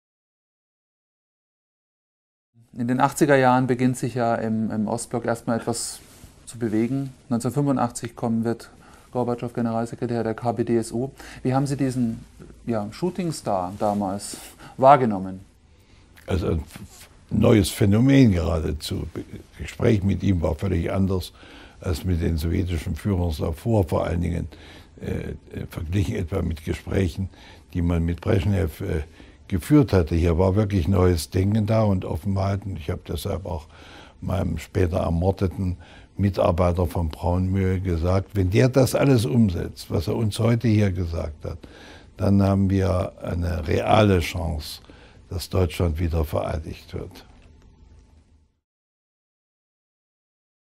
Hans-Dietrich Genscher: Michail Gorbatschow als "neues Phänomen"
Hans-Dietrich Genscher, damals Außenminister der Bundesrepublik, spricht von einem "neuen Denken" in Moskau, das die deutsche Wiedervereinigung erst möglich gemacht habe.